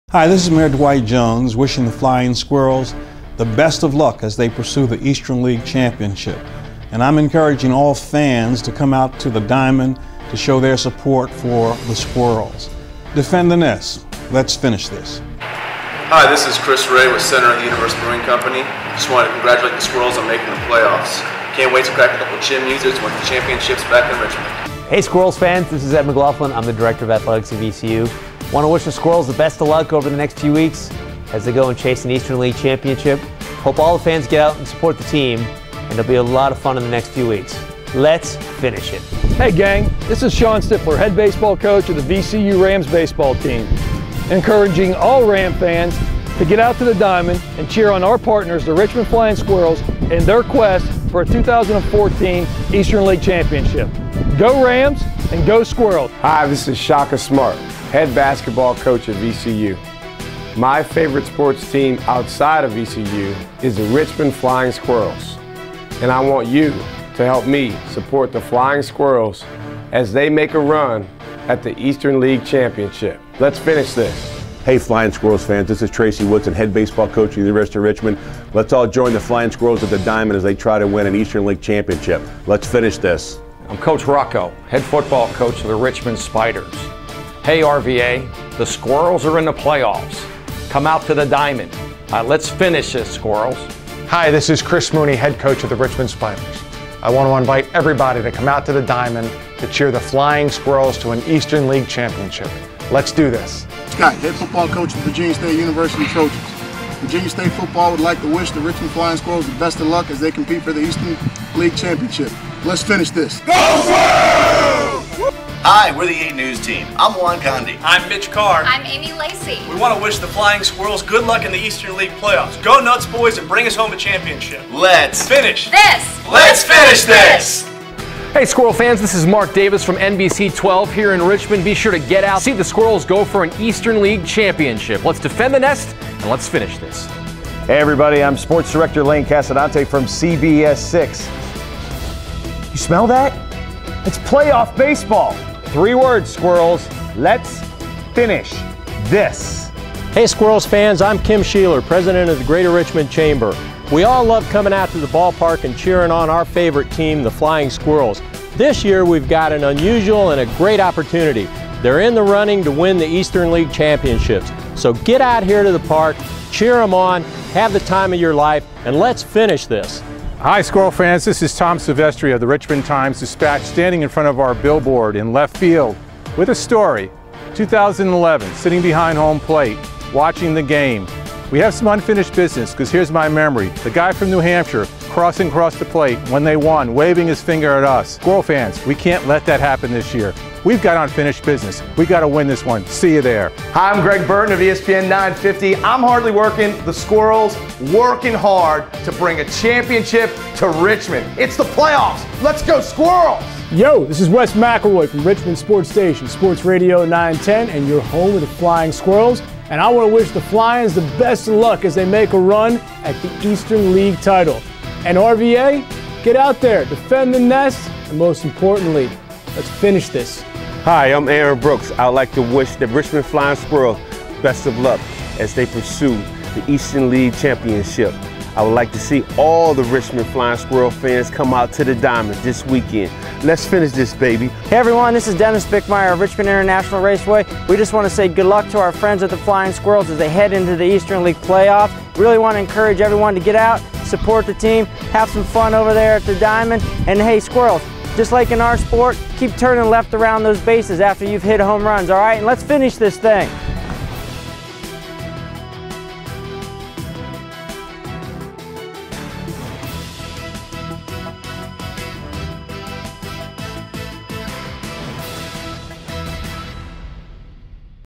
2014 Flying Squirrels Playoff Promotion
Prior to the 2014 Eastern League Western Division Championship Series, the Richmond Flying Squirrels visited local celebrities throughout Richmond to help Defend the Nest throughout the playoffs.